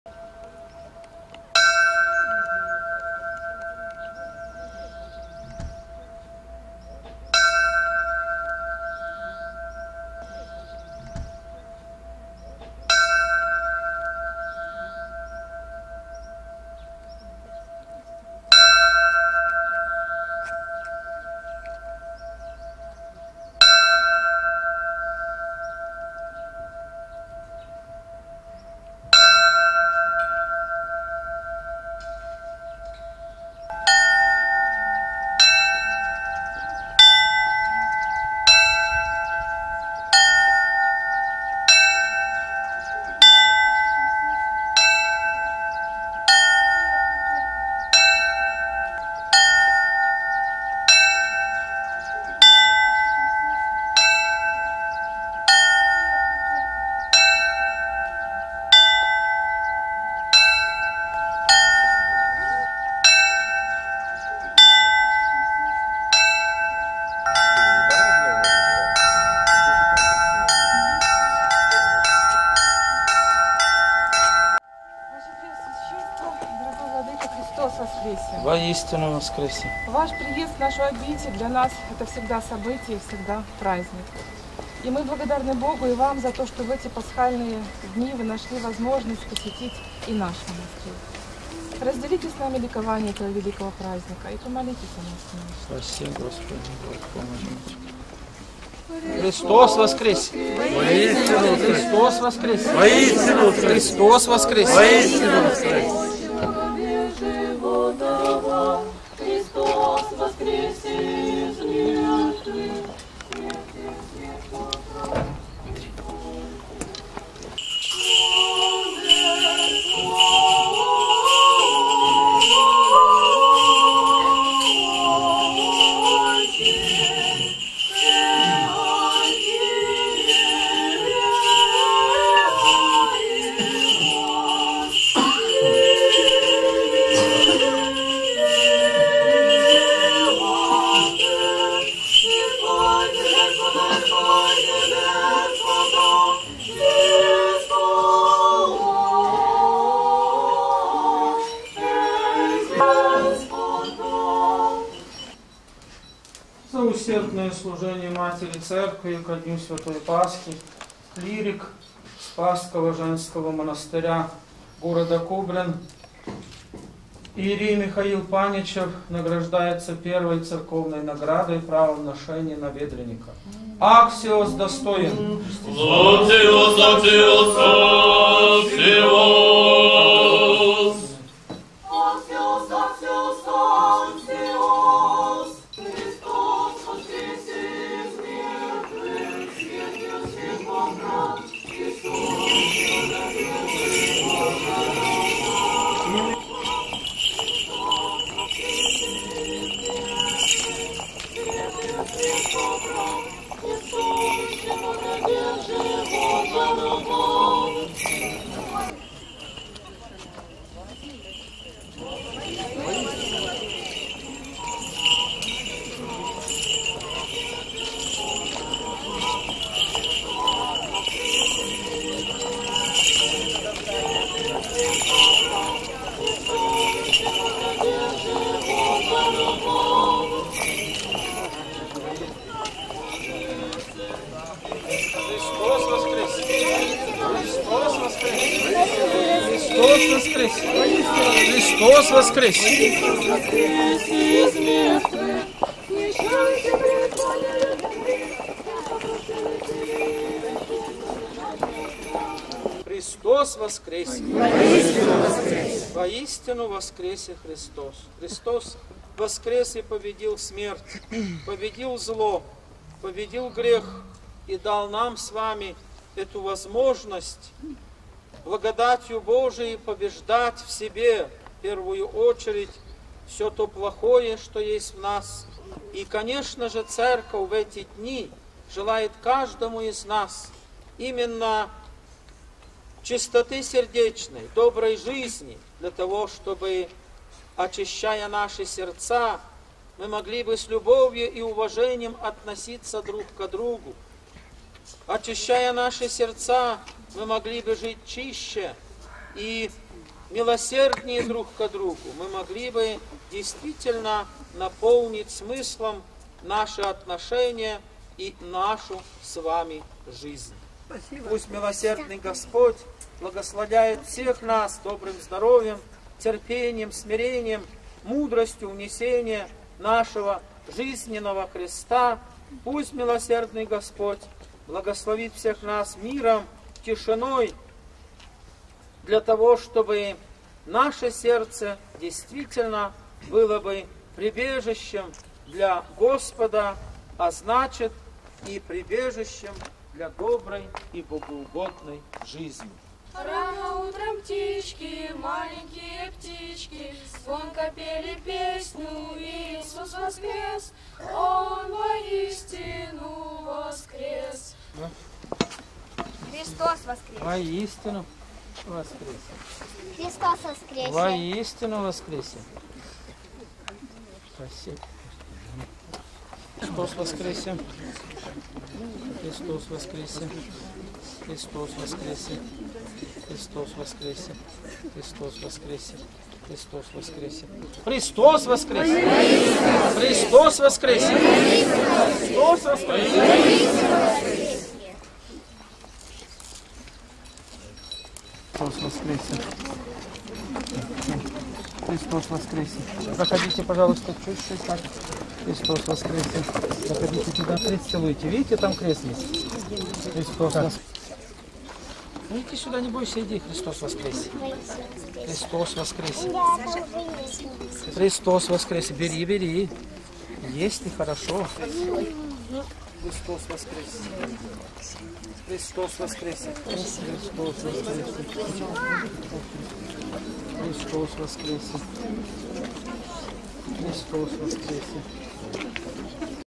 Пасхальная вечерня в Спасском женском монастыре г. Кобрина
Пасхальная вечерня в женском монастыре в честь Всемилостивого Спаса в г. Кобрине, архиерейским чином.

22 апреля 2014 г.,